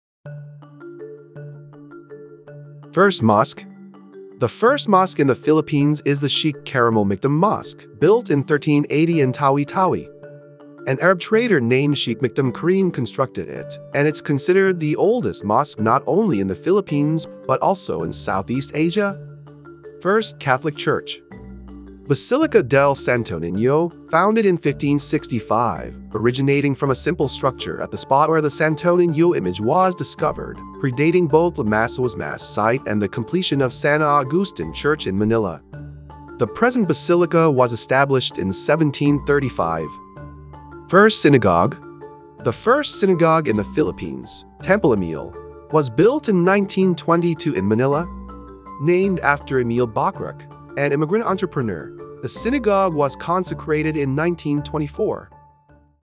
Religious Milestones in the Philippines: Mosques, Churches, Synagogues, and Indigenous Faith
First mosque
The first mosque in the Philippines is the Sheikh Karimul Makhdum Mosque, built in 1380 in Barangay Tubig Indangan, Simunul, Tawi-Tawi. An Arab trader named Sheikh Makhdum Karim constructed it, and it's considered the oldest mosque not only in the Philippines but also in Southeast Asia.
First Catholic church:
Basilica del Santo Niño
Founded in 1565: Originating from a simple structure at the spot where the Santo Niño image was discovered, predating both Limasawa's Mass site and the completion of San Agustin Church in Manila. The present basilica was established in 1735.

first synagogue
The first synagogue in the Philippines, Temple Emil, was built in 1922 in Manila. Named after Emil Bachrach, an immigrant entrepreneur, the synagogue was consecrated in 1924.